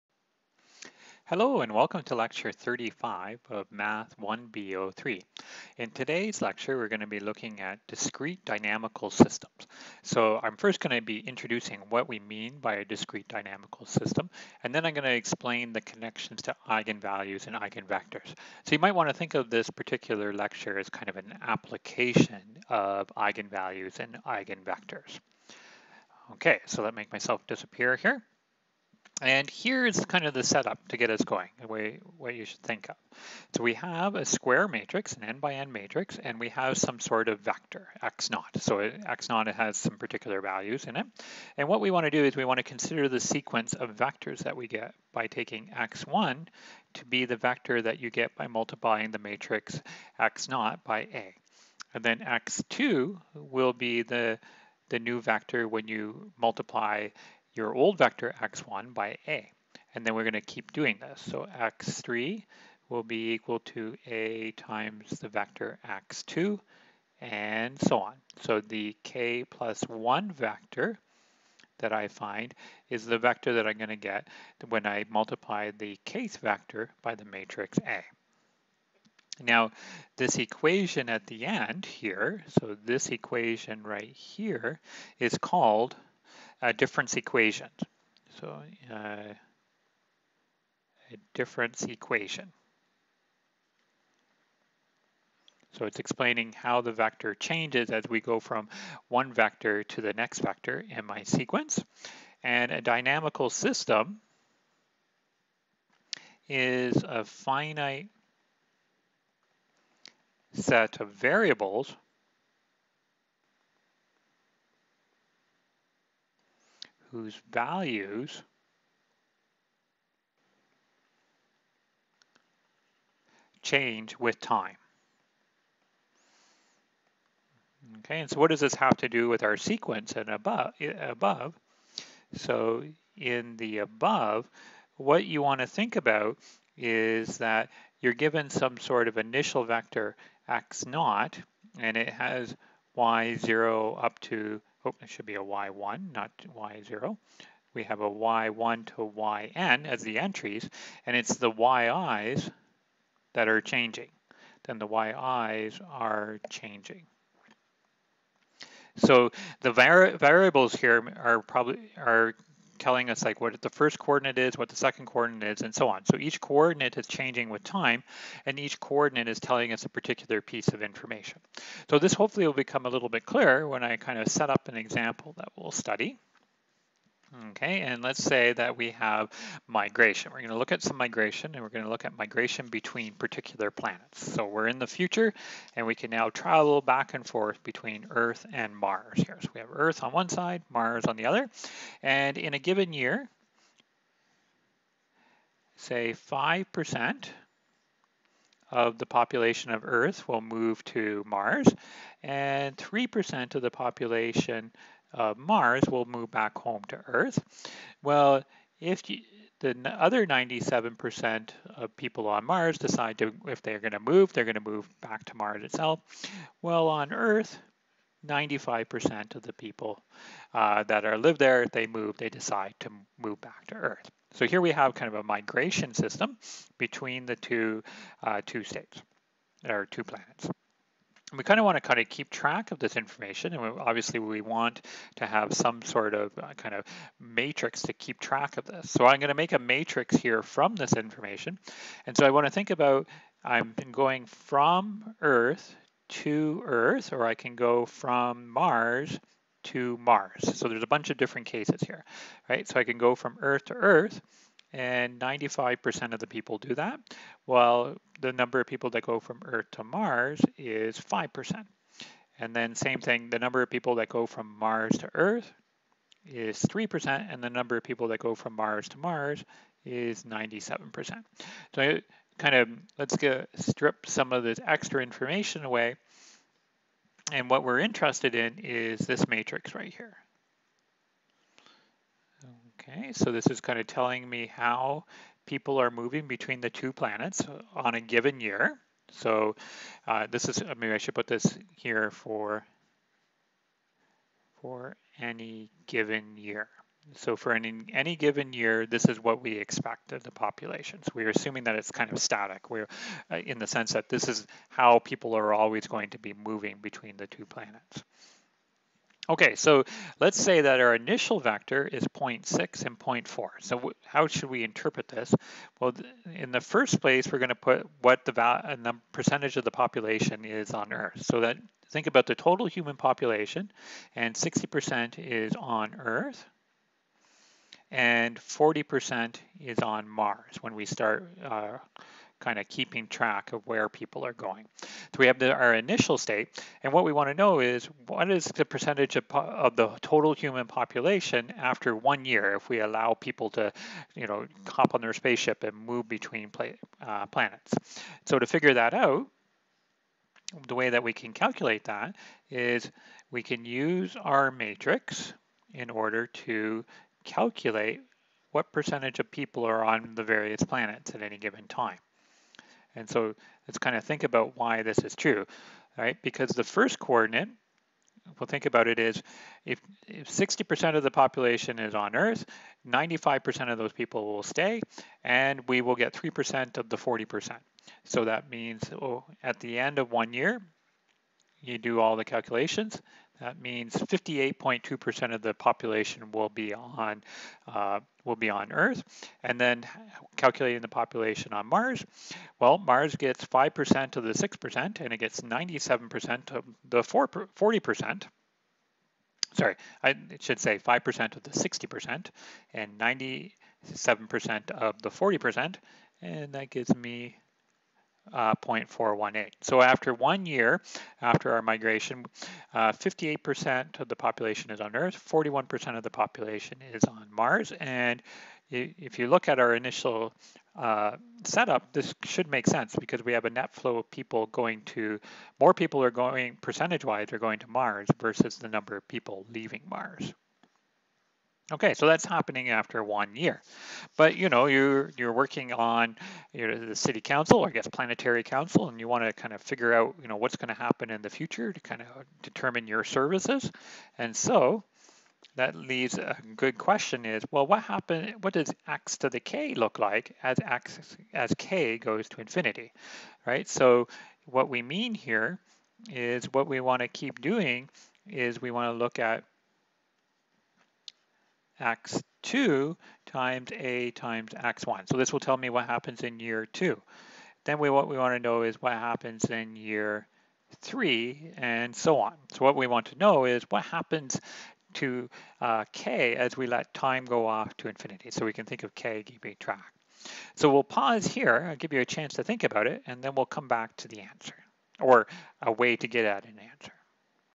Math 1B03 (2020-2021) Lecture 35 Part 1
In Lecture 35 we introduce dynamical systems.  In Part 1 we define dynamical systems, and given an example that is studied in the rest of the lecture.